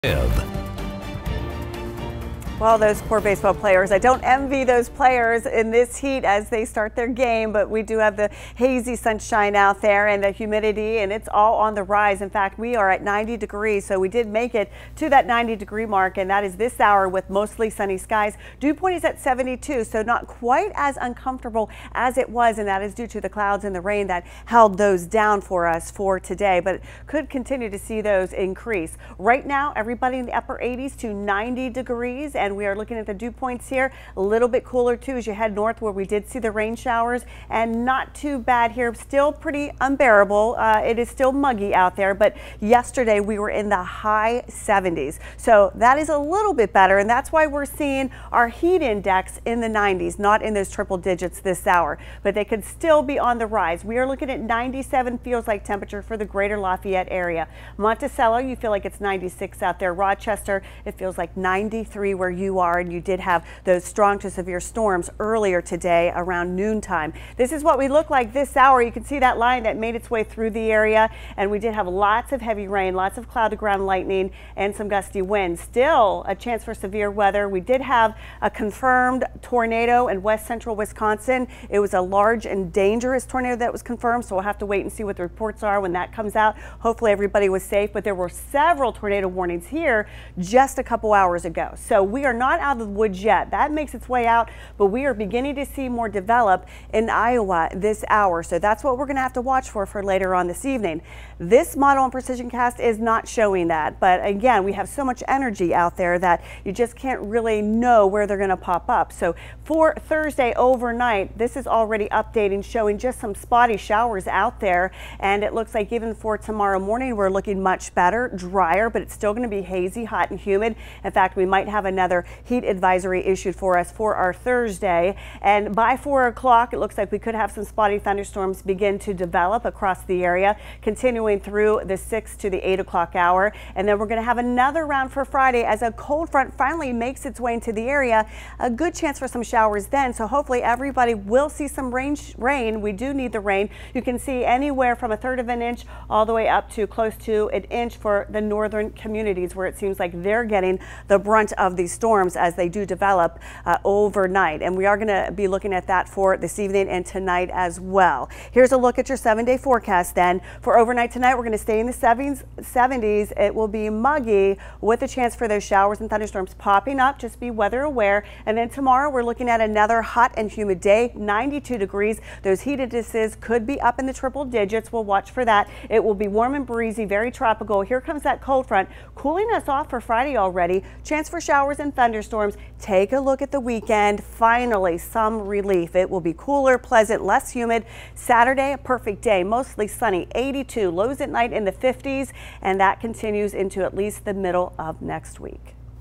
Precision18 Forecast (08/11/21)
Hot & steamy with another round of t'storms